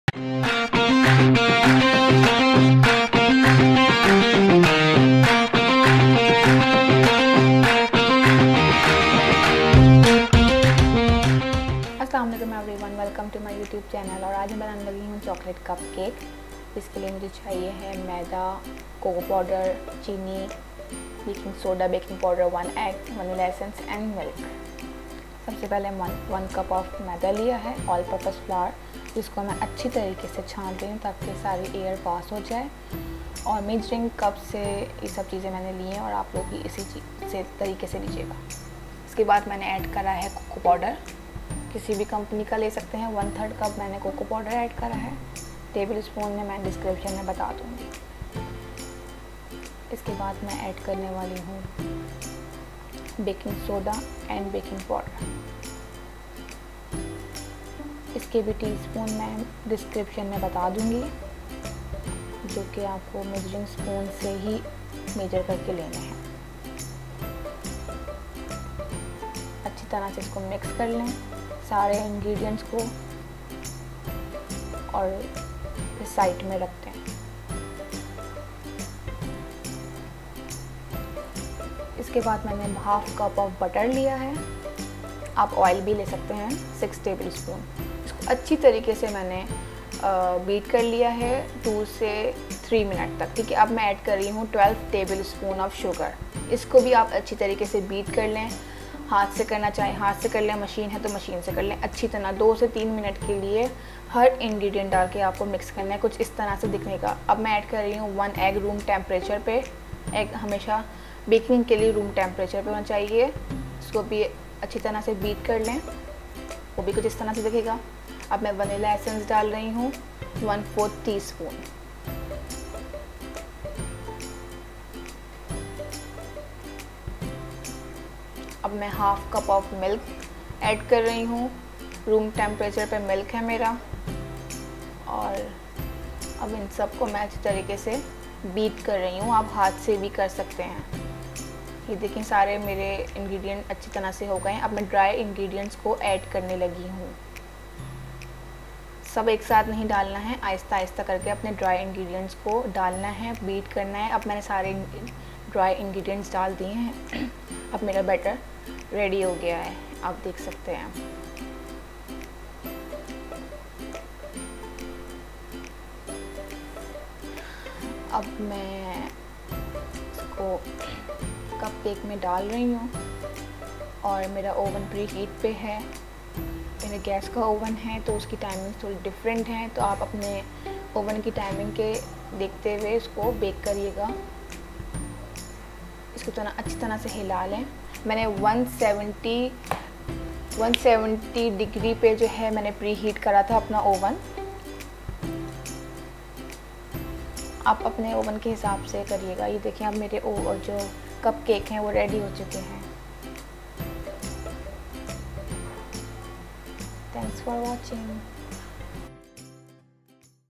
Chocolate Cupcake by Uroosa
Ingredients:
Chocolate Cupcakes:
Maida (All-purpose flour) 1 Cup
Cocoa powder 1/3 Cup(5 tbsp and 1 tsp
Baking powder ¼ tsp
Baking soda ¾ tsp(1/4tsp 3 times)
Makhan (Butter) Half Cup
Caster sugar ¾ Cup(12 Tbsp)
Anda (Egg) 1 room temperature
Vanilla essence ½ tsp
Doodh (Milk) warm Half Cup